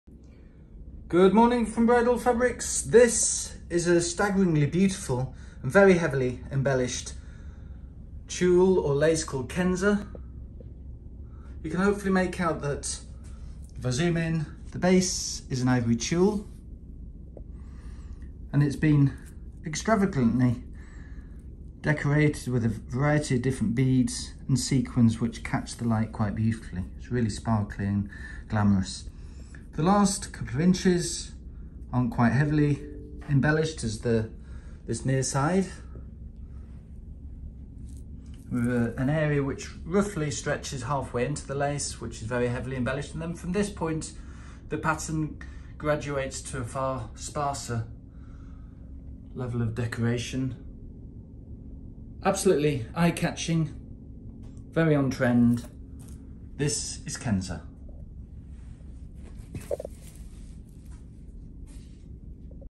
Ivory Sequined & Beaded Tulle - Kenza
An extremely heavily sequin and bead decorated bridal Tulle , that measures 132 cm (52") wide. This is a wonderfully sophisticated product with beads and sequins that are graduated in density to produce a wonderful effect.

This striking design is now also available in Silver.

We offer a very similar product as Vitality and Sensilla.